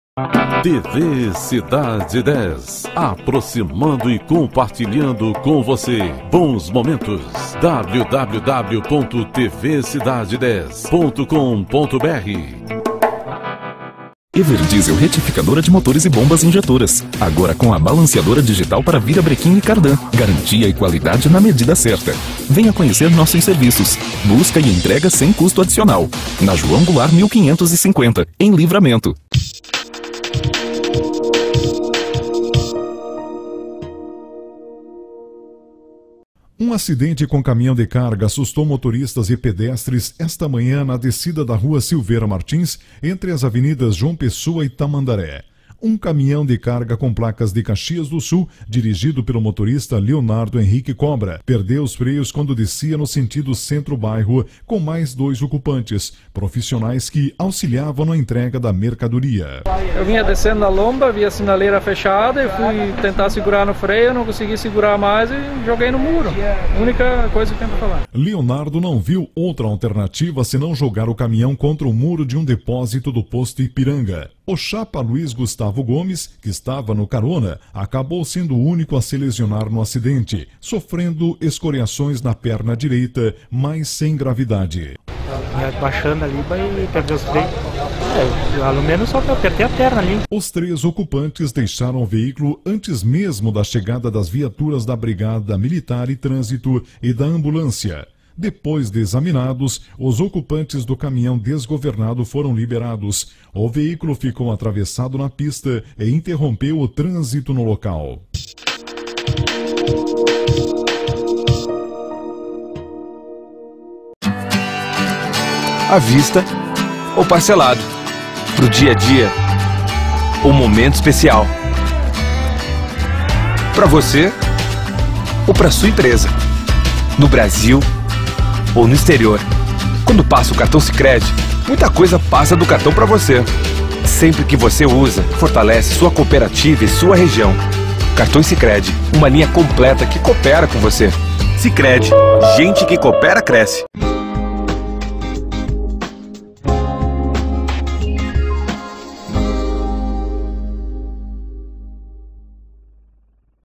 TV Cidade 10 Livramento, Valorizando o que é nosso!!!
Motorista joga caminhão contra muro na Rua Silveira Martins